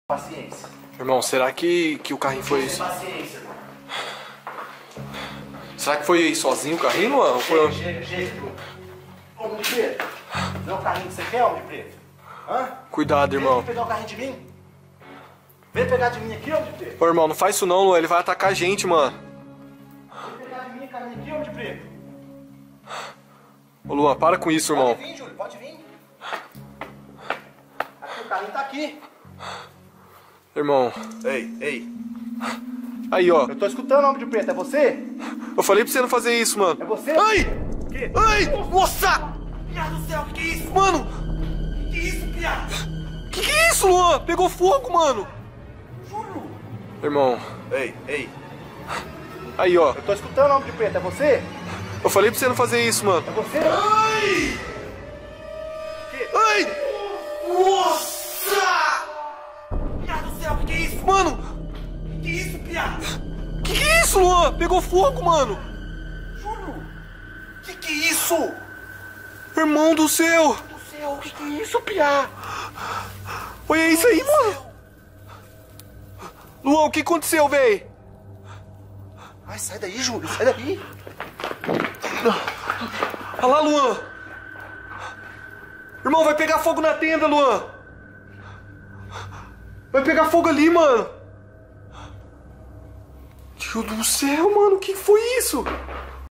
DEMÔNIO SE MANIFESTA NO CARRINHO DE BEBÊ FANTASMA
Carrinho do bebê fantasma pega fogo devido manifestação demoníaca.

⨷ ATENÇÃO ⨷
Esse NÃO É o canal OFICIAL do LUAN MENDES. Esse é um canal feito de fã para fãs destacando os melhores momentos dos vídeos.